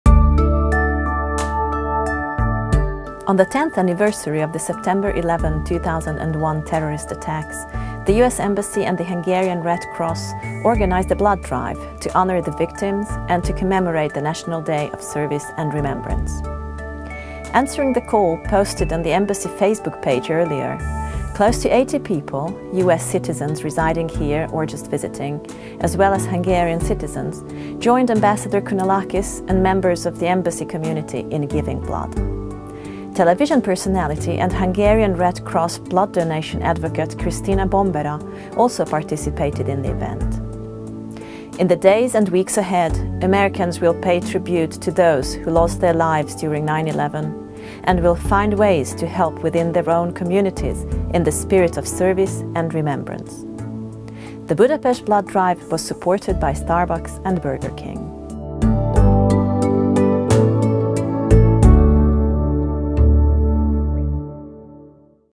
Embassy 9/11 Memorial Blood Drive
On the tenth anniversary of the September 11, 2001 terrorist attacks, the U.S. Embassy and the Hungarian Red Cross organized a blood drive to honor the victims and to commemorate the National Day of Service and Remembrance.

Answering the call posted on the Embassy Facebook page earlier, close to 80 people - U.S. citizens residing here or just visiting, as well as Hungarian citizens - joined Ambassador Kounalakis and members of the Embassy community in giving blood. Television personality and Hungarian Red Cross blood donation advocate Krisztina Bombera also participated in the event.

In the days and weeks ahead, Americans will pay tribute to those who lost their lives during 9/11 and will find ways to help within their own communities in the spirit of service and remembrance.

The Budapest blood drive was supported by Starbucks and Burger King.